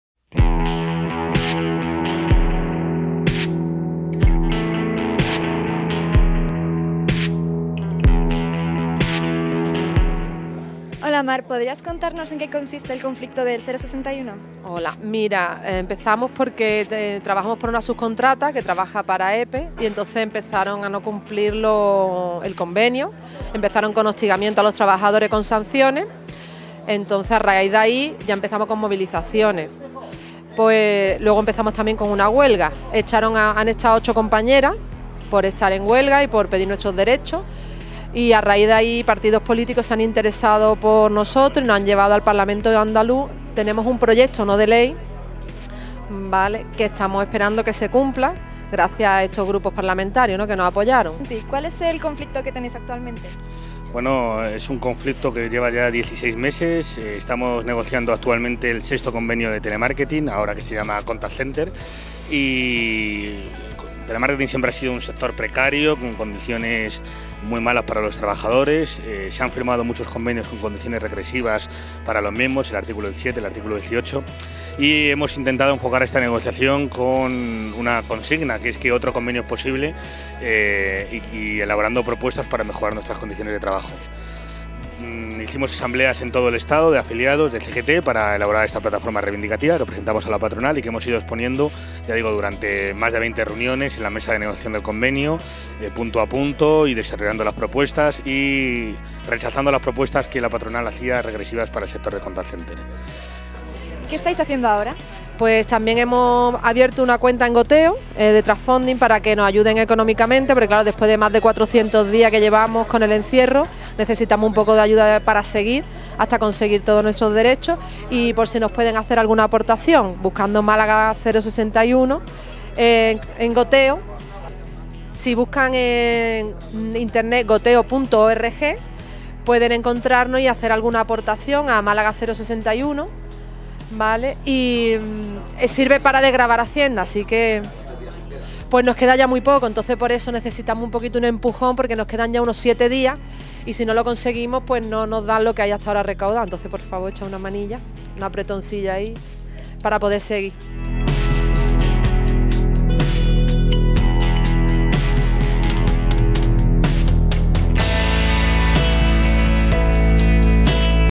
Conflictos del 061 en Telemarketing CGT
Lxs compañerxs de CGT Telemarketing nos explican los diferentes conflictos en los que están involucrados en este momeneto